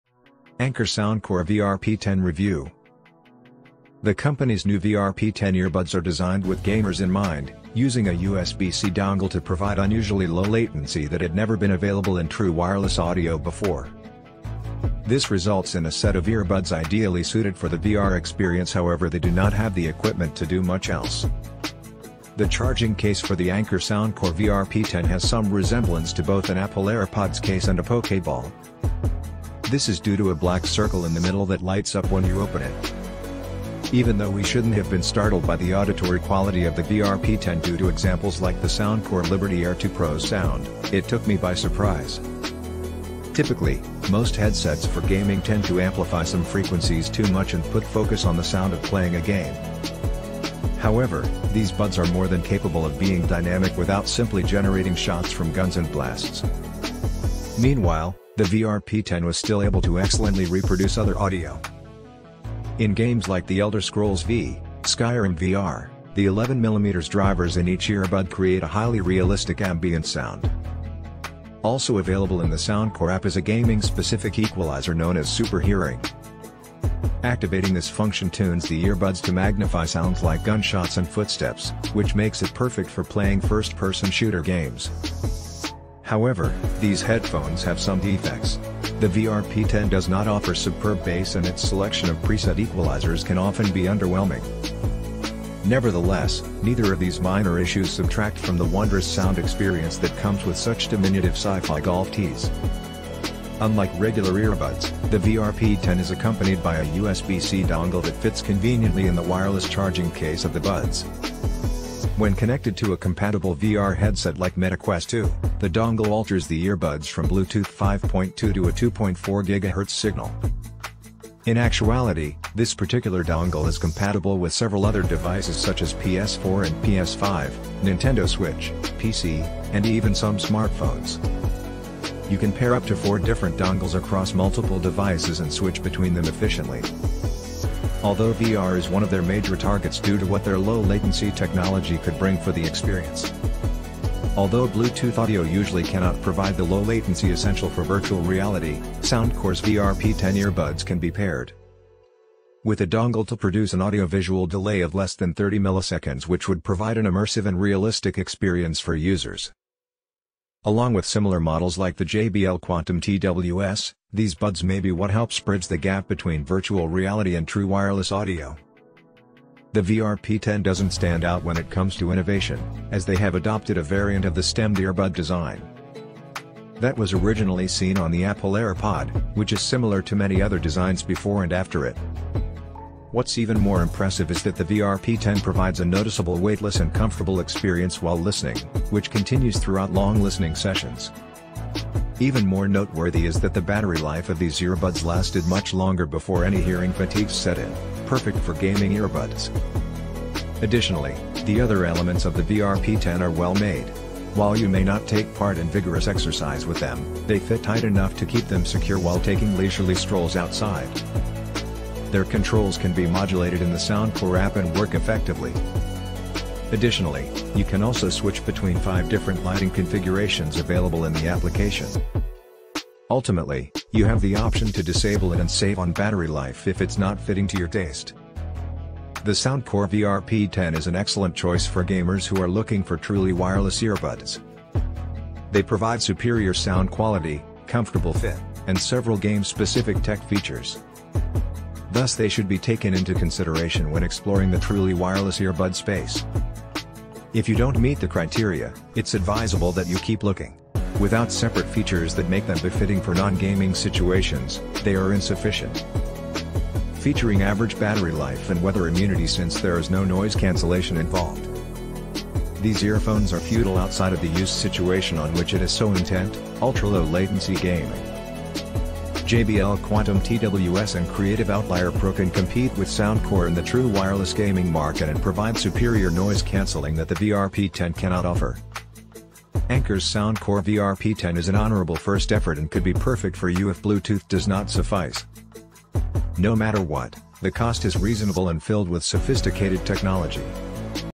Anker Soundcore VR P10 Review | Gaming Earbud
Amazon Product Name : Anker Soundcore VR P10 Review

Looking for a gaming earbud that delivers exceptional audio quality and comfort? Check out our review of the Anker Soundcore VR P10 Gaming Earbuds! With powerful drivers and a unique design that fits snugly in your ear, these earbuds will immerse you in your favorite games like never before. We'll go over all the features and benefits of the Anker Soundcore VR P10, so you can decide if they're the right choice for your gaming needs. Don't miss out on this in-depth review!